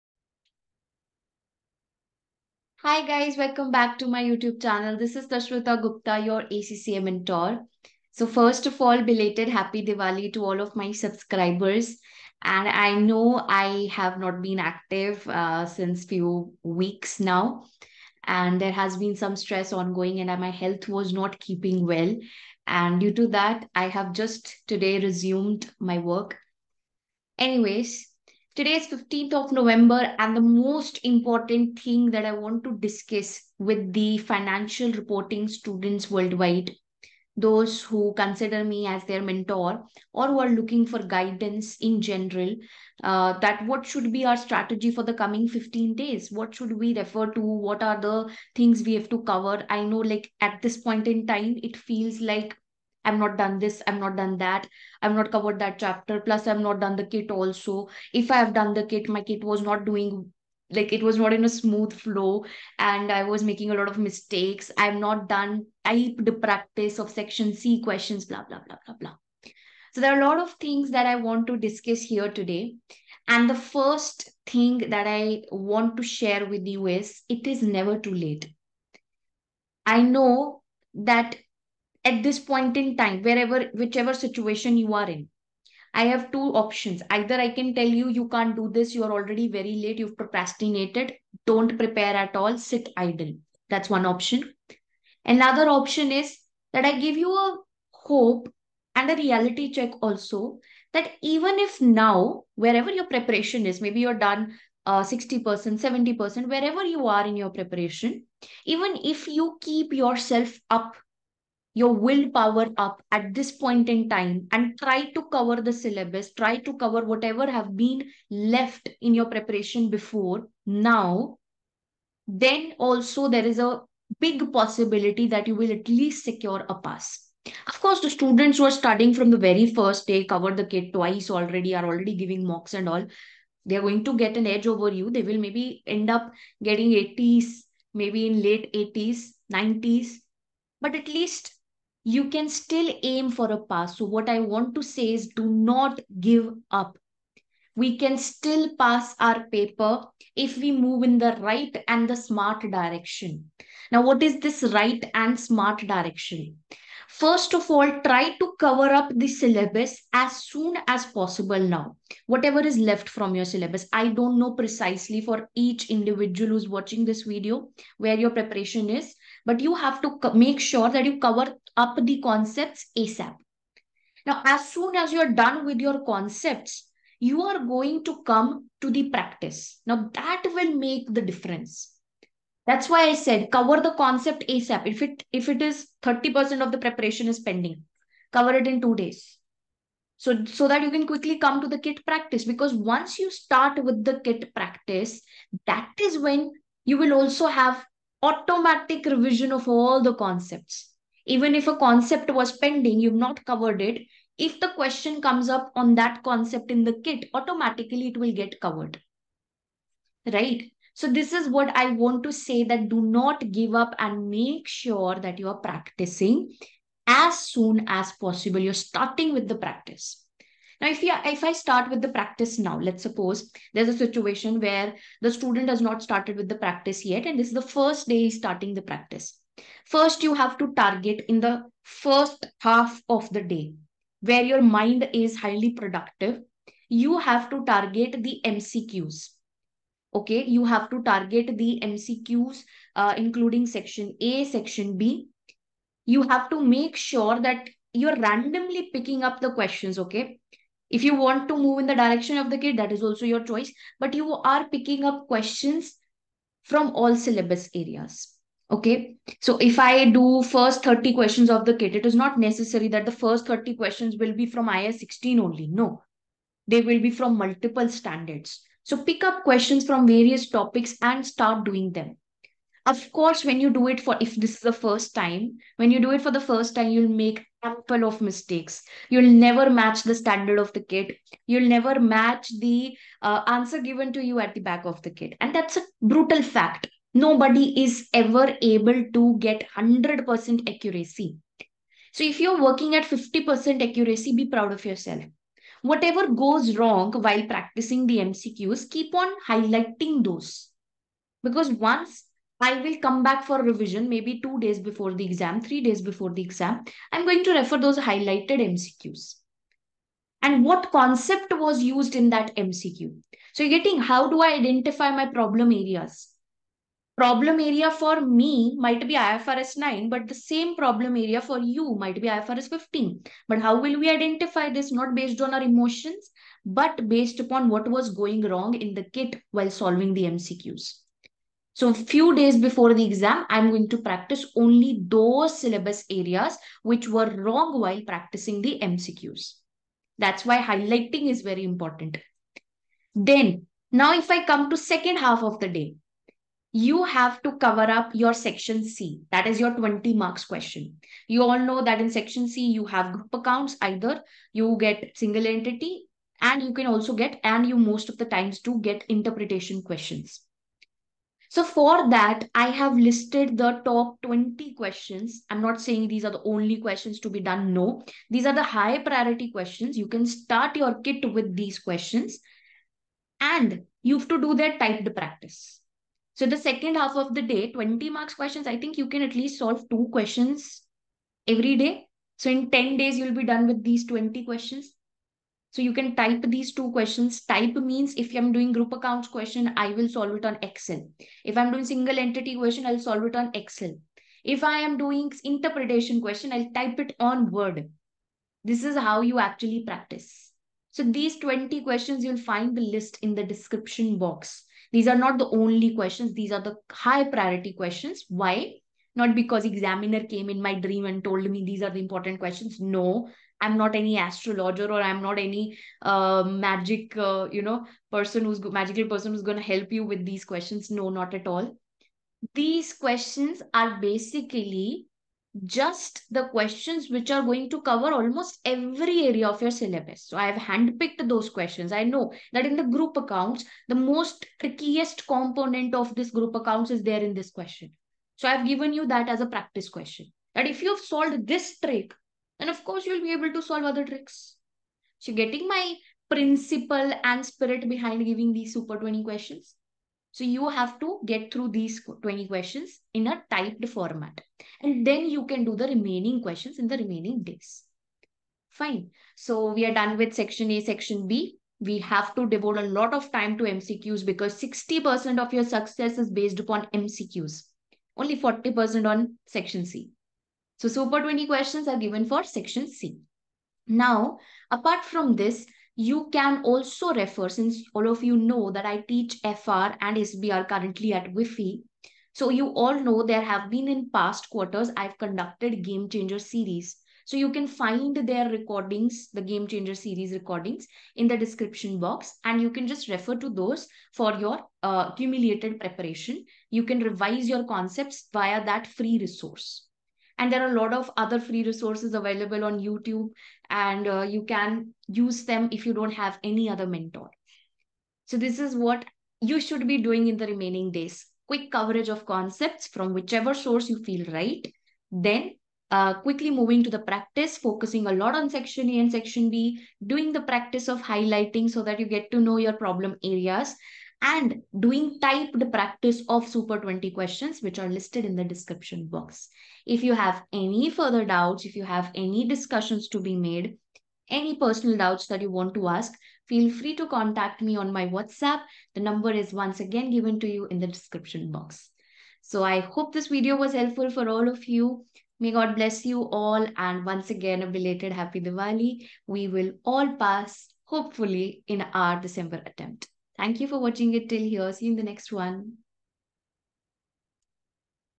ACCA FR Exam Strategy: 15-Day Plan || Super 20 Questions for Success
SUPER 20 QUESTIONS FROM KAPLAN KIT
1. FRESCO
2. MOBY
3. KANDY
4. MOSTON
5. MIMS CO.
6. PREMIER
7. PANDAR
8. PYRAMID
9. VIAGEM
10. PLASTIK
11. PLANK
12. CHANG
13. HYDAN
14. YOGI
15. PITCARN
16. FUNJECT
17. PASTRY
18. FIT
19. VERNON
20. LOUDON